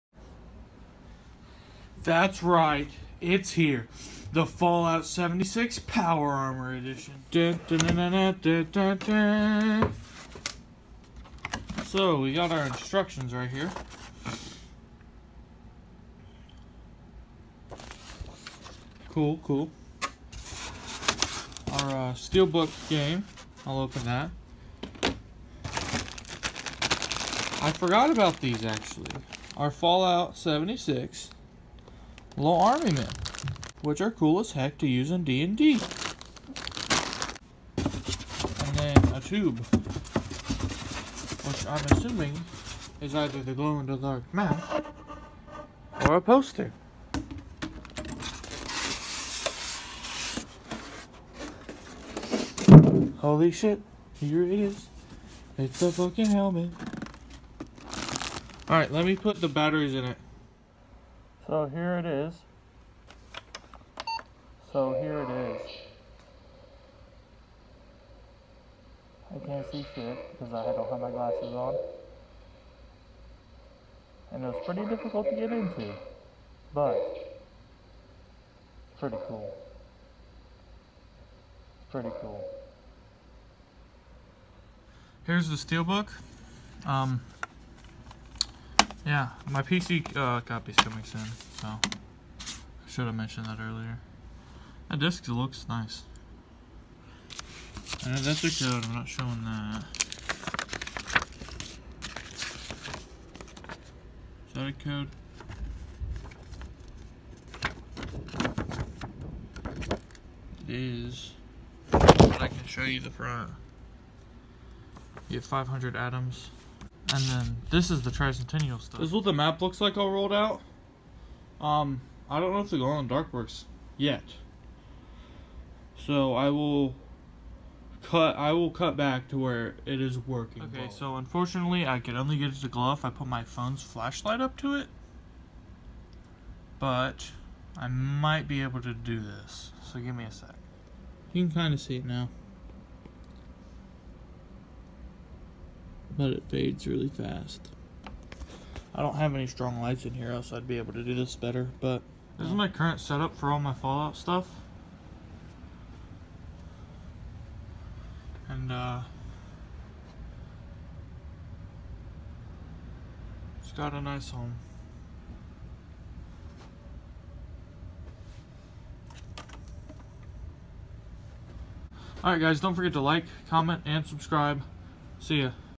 Fallout 76 Power Armor Edition Unboxing!
I got this early from my local GameStop. Thanks guys!